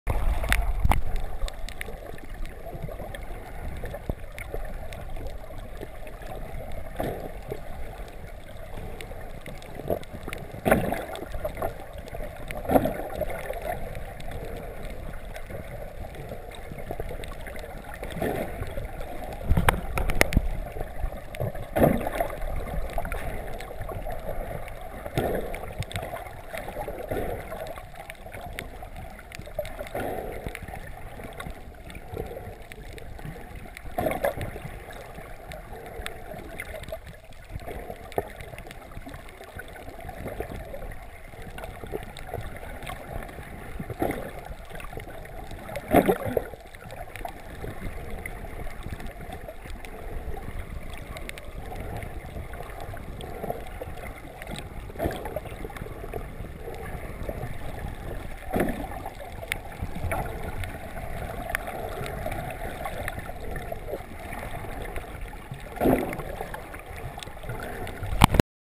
Morning before work
Dugong cruising in the shallows